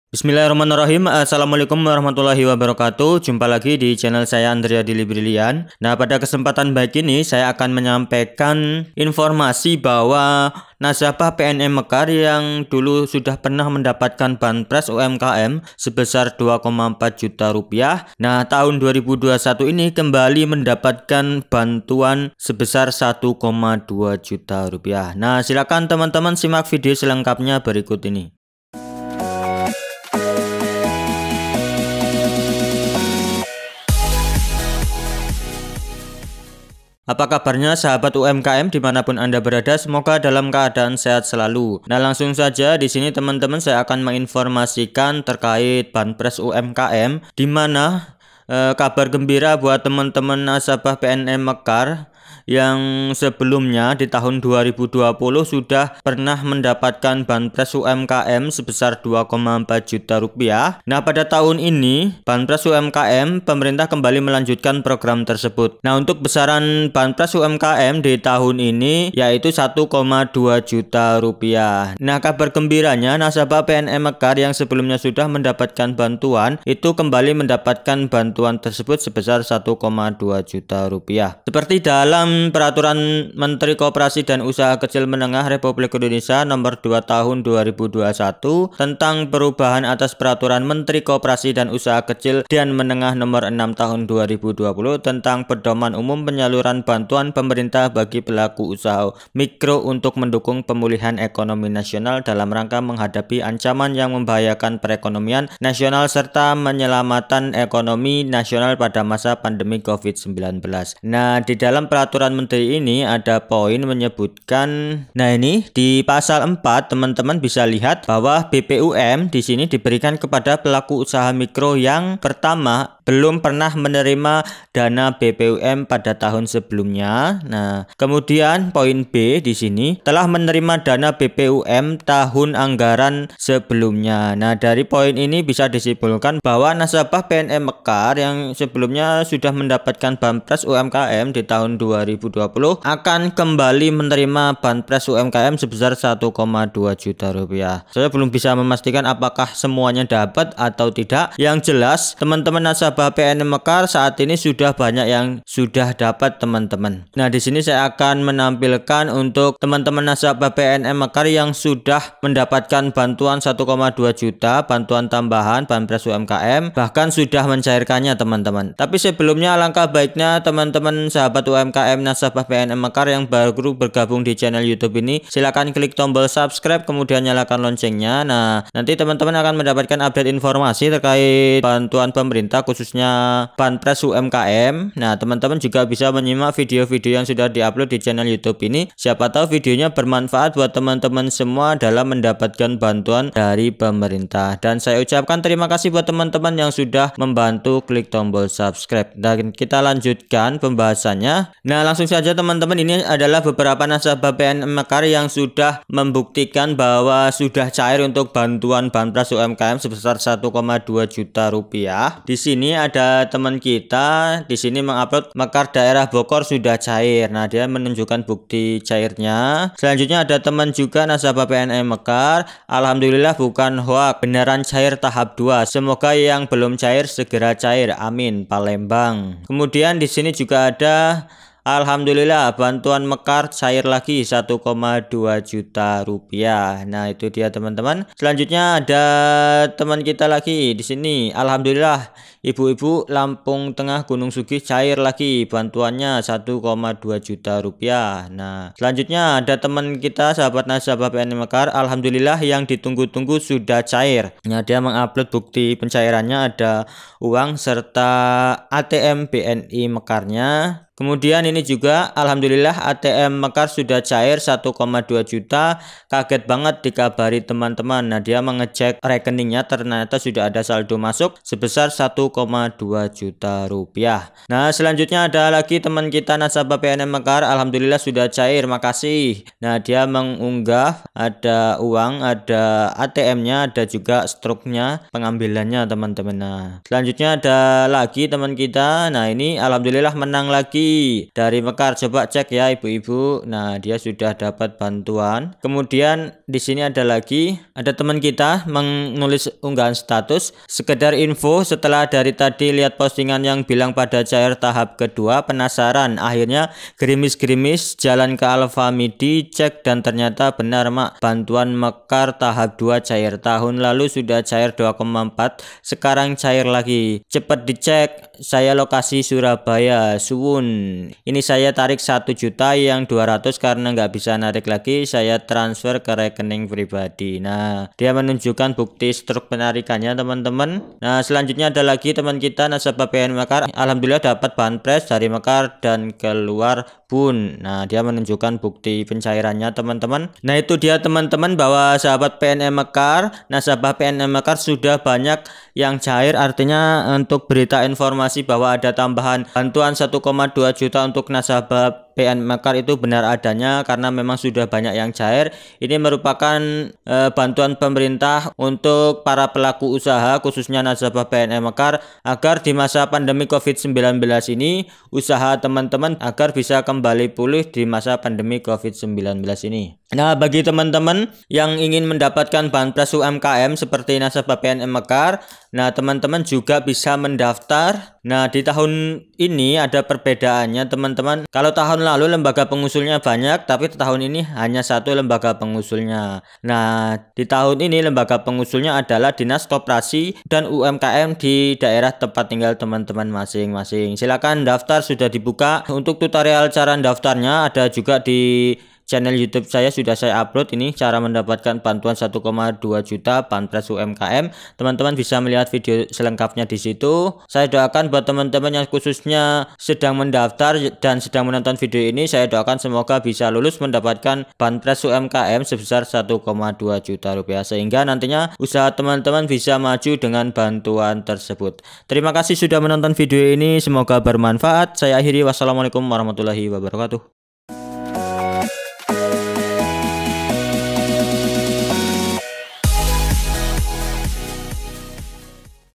NASABAH PNM MEKAR DAPET BANTUAN LAGI 1,2 JUTA
Assalamualaikum, Apakabar teman teman semua, kali ini saya membagikan informasi NASABAH PNM MEKAR DAPET BANTUAN LAGI 1,2 JUTA, Semoga Informasi ini bermanfaat buat teman-teman semua, Terimaksih Sudah Menonton, Semoga Bermanfaat